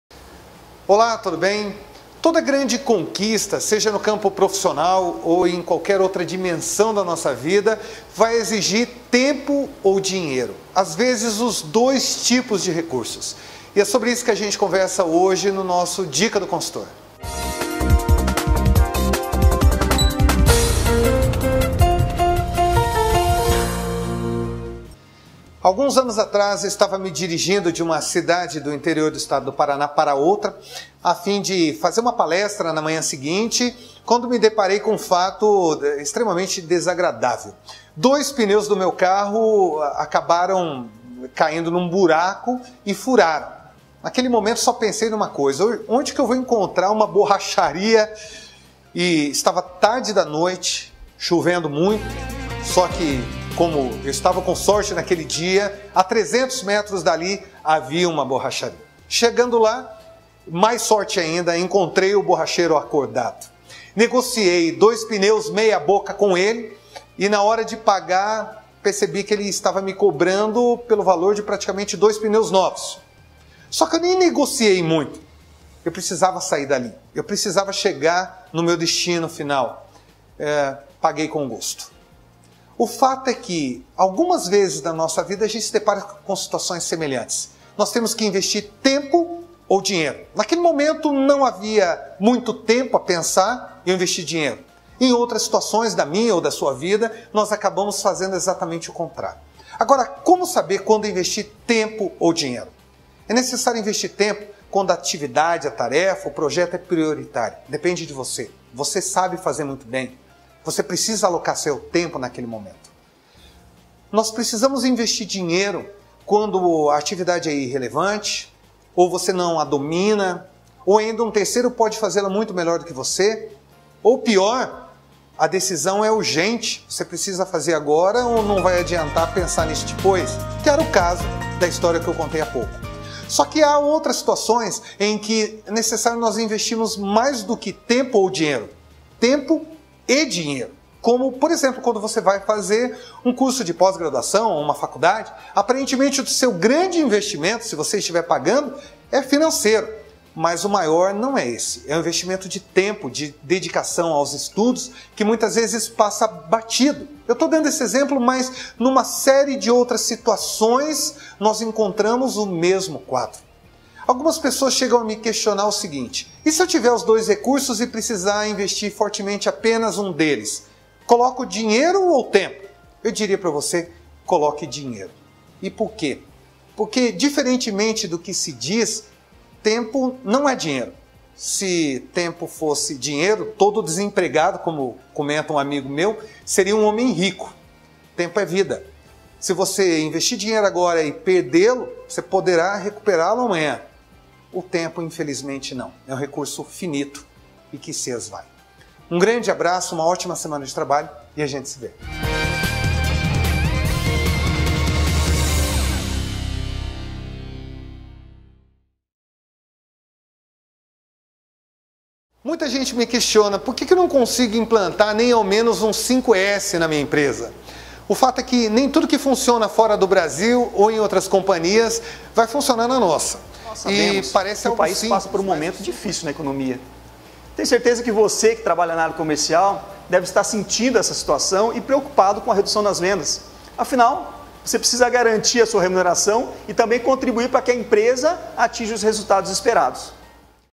Sucesso exige tempo e/ou dinheiro
Para o consultor Wellington Moreira obter sucesso exige tempo e/ou dinheiro. O segredo é saber utilizar esses recursos com sabedoria.